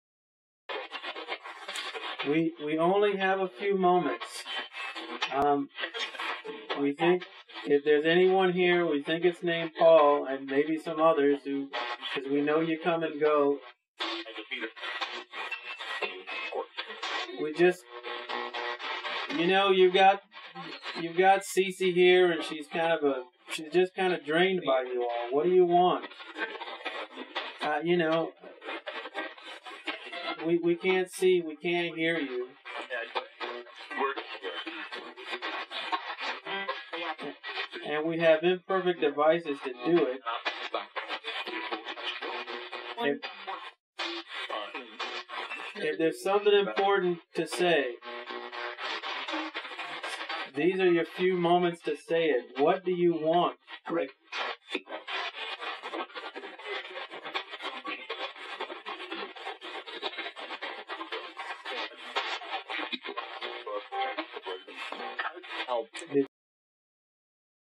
A Haunting in Anne Arundel County 2013/Jessup, Maryland ("Bring Us God's Help")
ITC ("Bring Us God's Help") captured by Maryland Paranormal Research ® at a private residence in Jessup, Maryland on 13 Jan 2013. Multiple possible entities provide parting pleas and shots. One asks ("Bring Us God's Help").  The P-SB7 was set for the AM band with a 250 ms reverse sweep rate. Audio was captured on a Zoom H2N Handy Recorder, Mackie 402-VLZ3 Mixer and two BEHRINGER XM1800S Dynamic Microphones. [AUDIO ENHANCED][HEADPHONES RECOMMENDED]